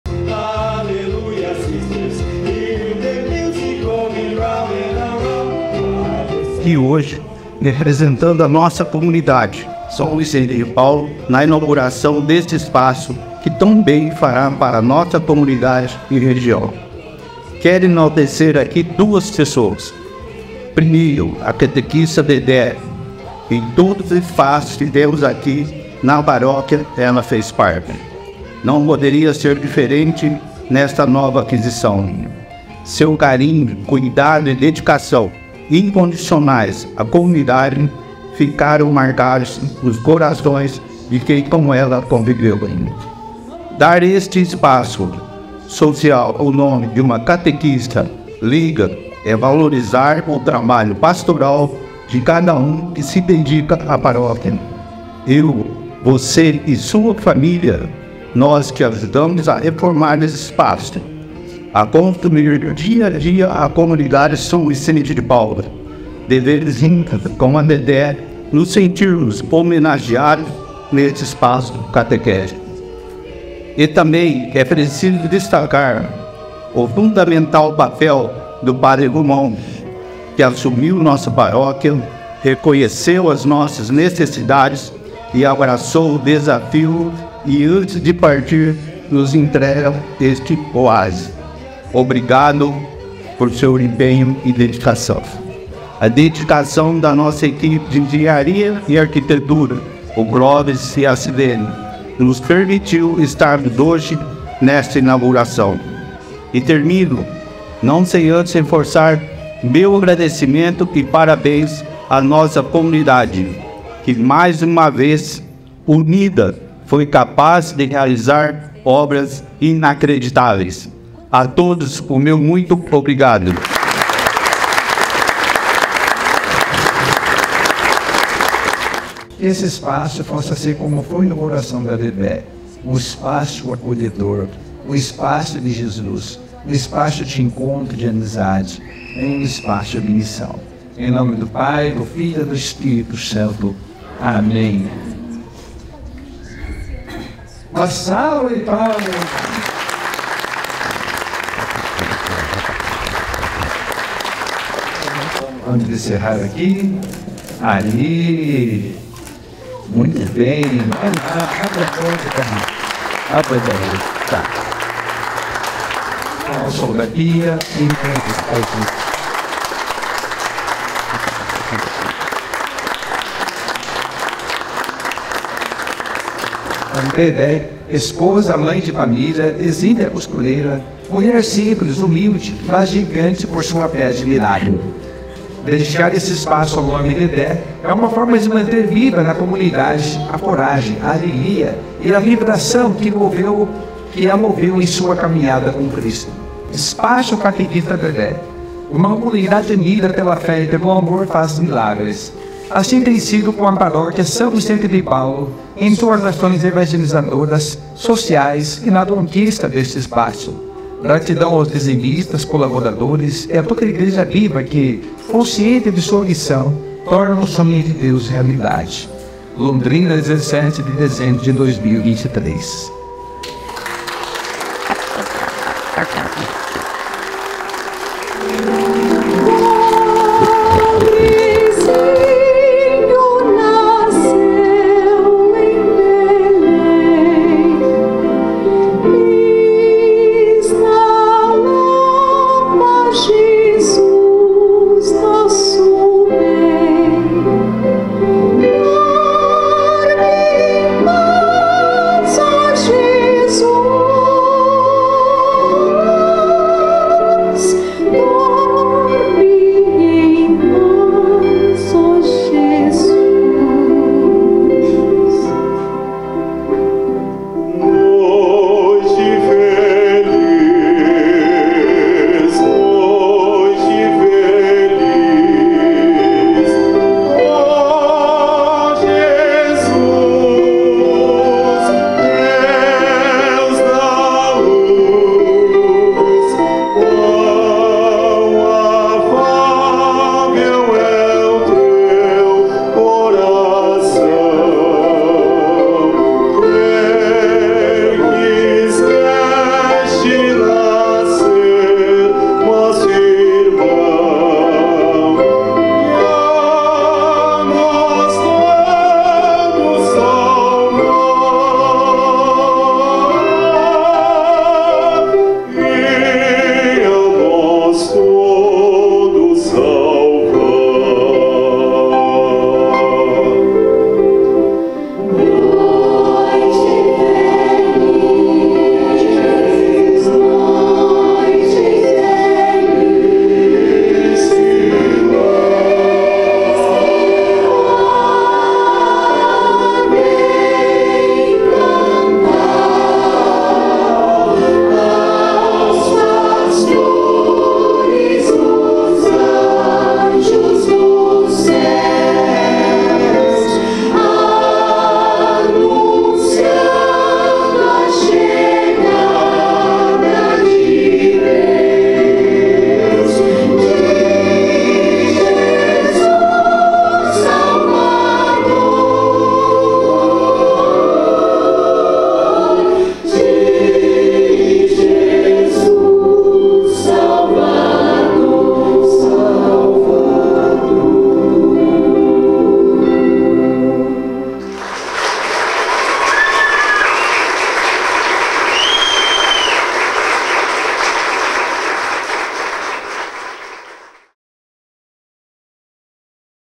INAUGURAÇÃO DO ESPAÇO CATEQUISTA DEDÉ
Inauguração das obras de reforma do Espaço Catequista Dedé. 17/12/2023.